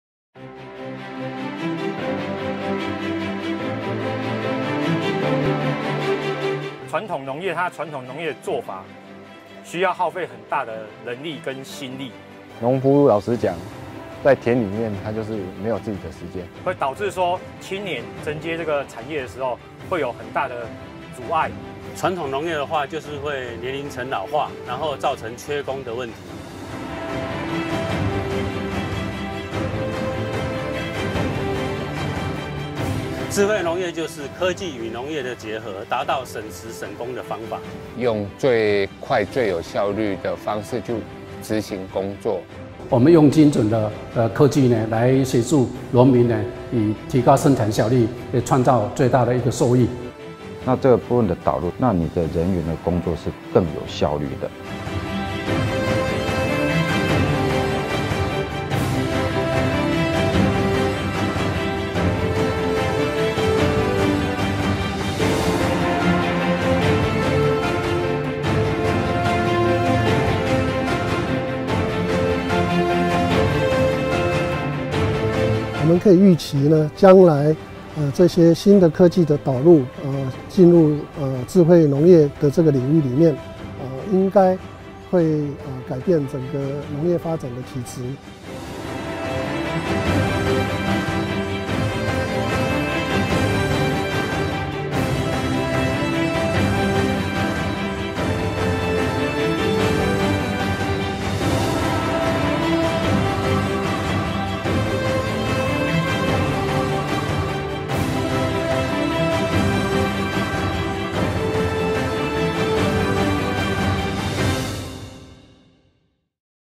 Smart Agriculture in Taiwan
Agriculture is transitioning from a traditional labor-intensive sector to one with automation and smart digital management.

With the assistance of technology and digitalization, production capacity is being upgraded while working hours are being reduced to achieve maximum efficiency.